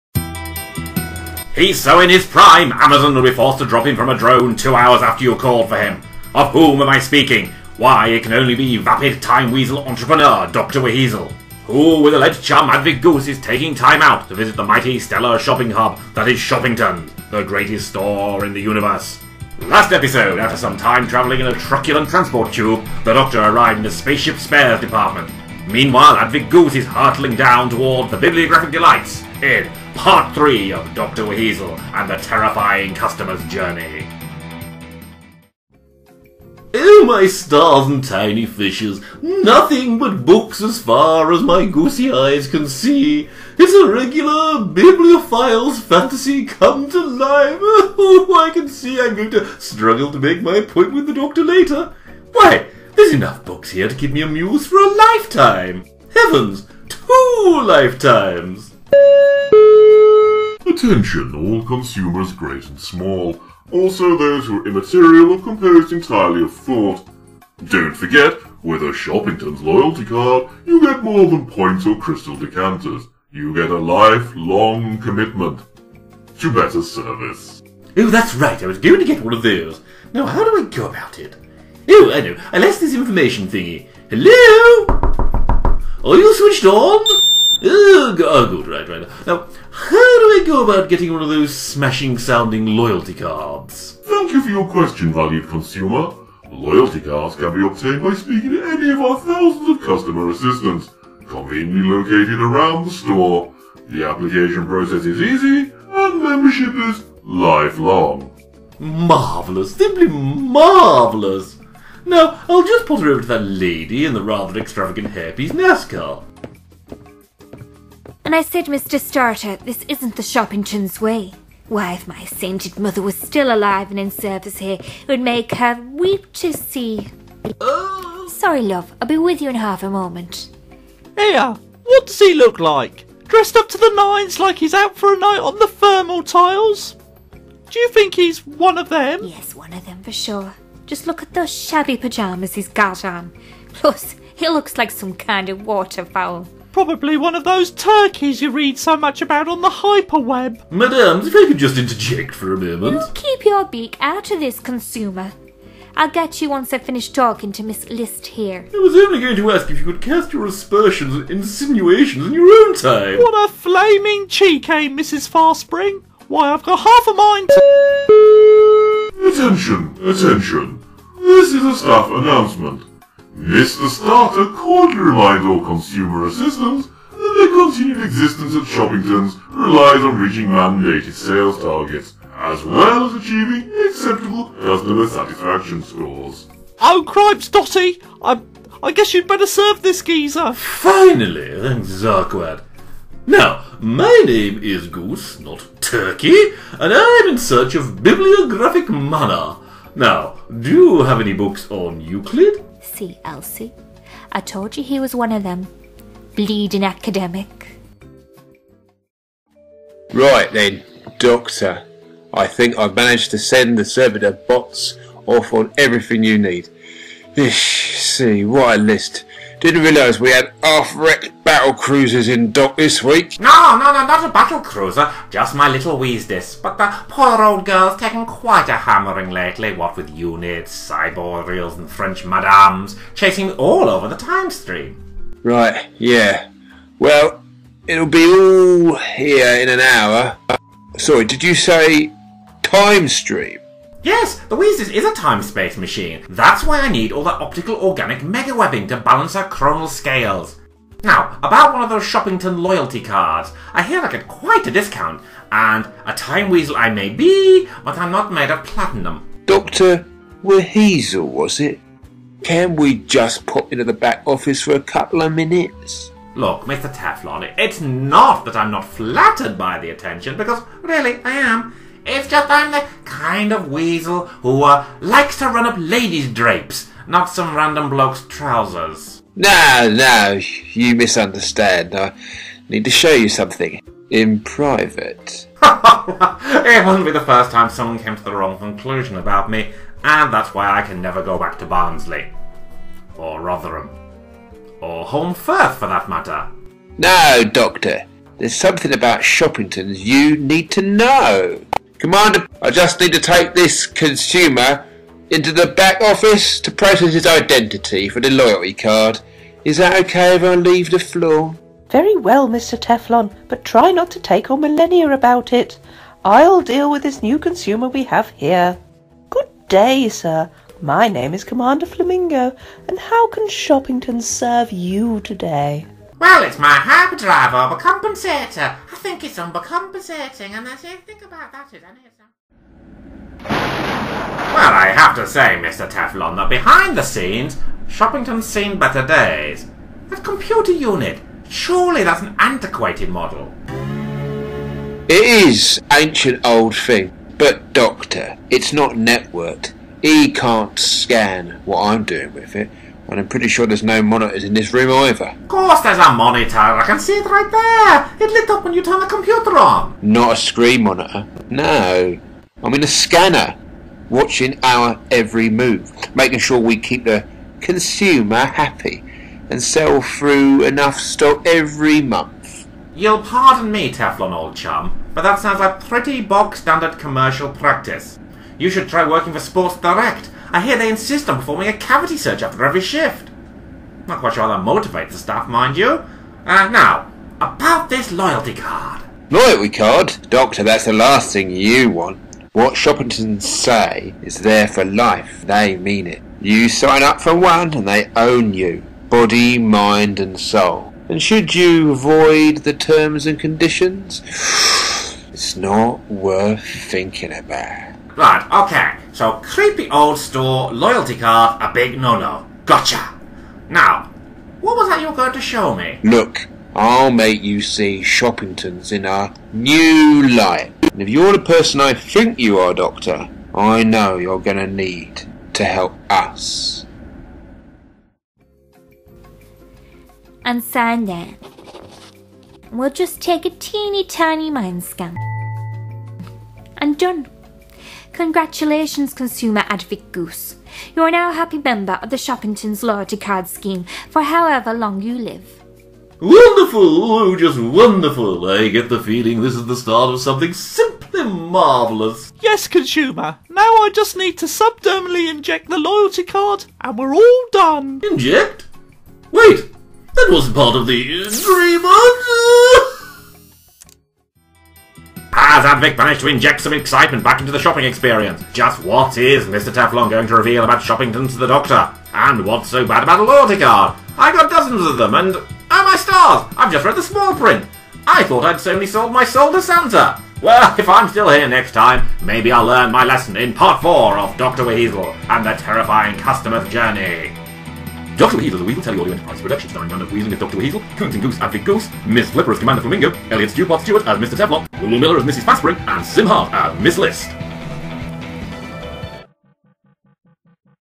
Doctor WHeasel & the Terrifying Customer's Journey (Part 3)
As the Doctor and Advick shop their little hearts out at Shoppingtons', the greatest shop in the universe.  But does something sinister lurk at the heart of this premier commercial experience?